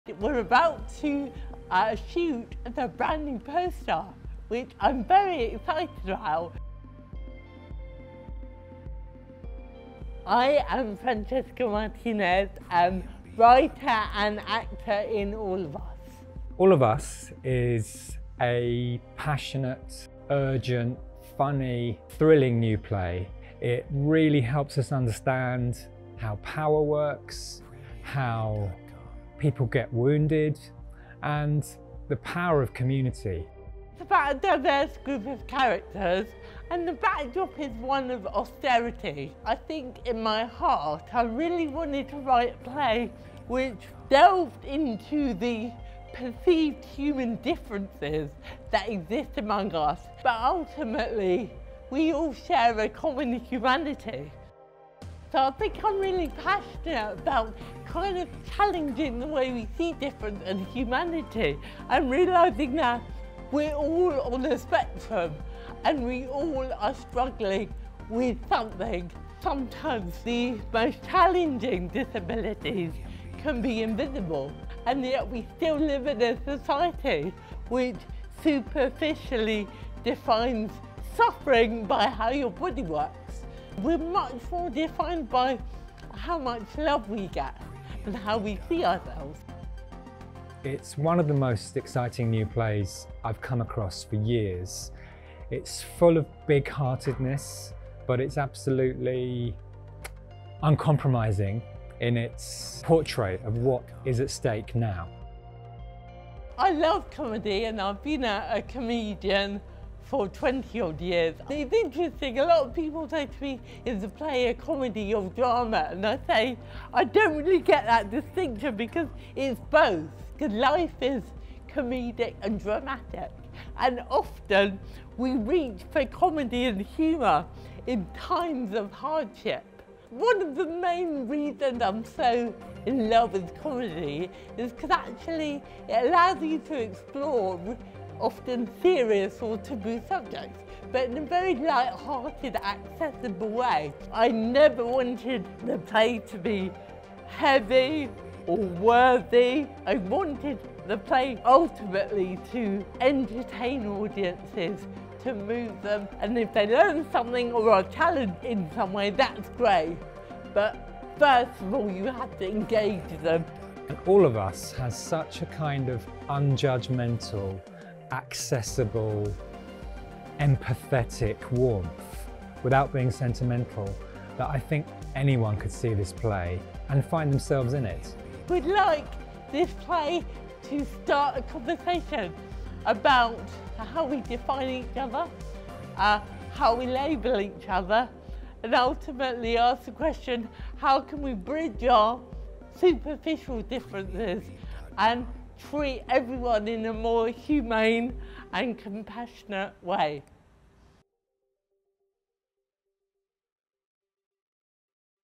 “Life is comedic and dramatic.”️️ ❤️ Francesca Martinez and Ian Rickson introduce new play All of Us
Award-winning comedian, actor and writer Francesca Martinez and director Ian Rickson introduce new play All of Us, coming to the Dorfman theatre this summer

‘I really wanted to write a play that delved into the perceived human differences that exist among us.’ - Francesca Martinez
Jess (Martinez) has a great life: a job she loves, a sharp sense of humour and a close group of friends.

When austerity threatens the world that she has worked hard to build, Jess makes a stand to protect those she holds most dear.

Capturing the humour, sadness and joy of everyday life, All of Us is a passionate and timely look at the human cost of abandoning those who struggle to fit in.

National Theatre Live broadcast the best of British theatre to cinemas in the UK and around the world.